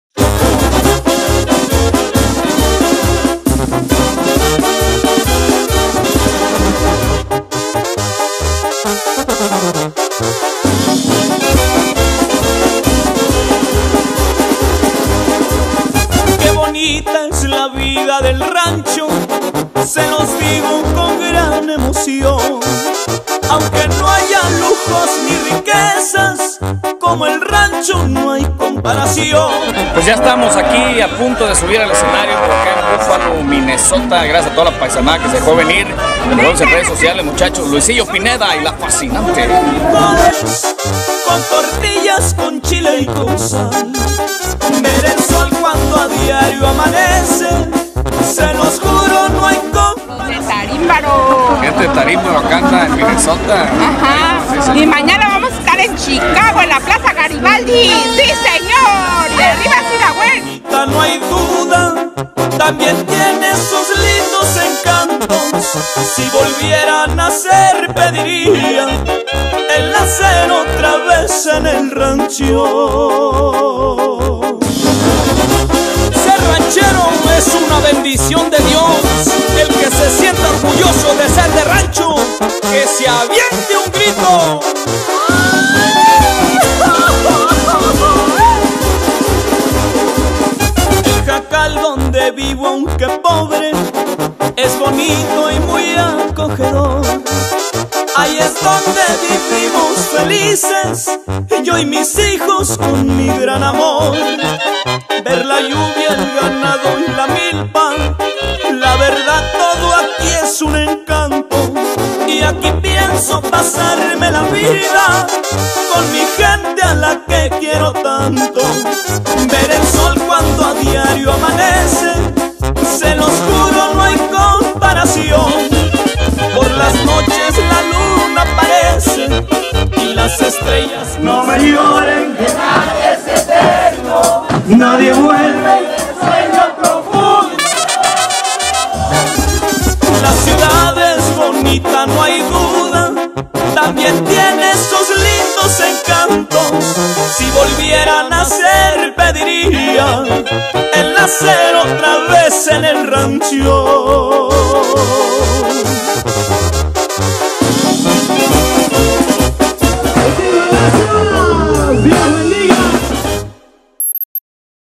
Luisillo Pineda Y La Fascinante del Merito Zirahuén (Desde Minnesota)
Un detrás de cámaras desde Buffalo, Minnesota en los Estados Unidos de América.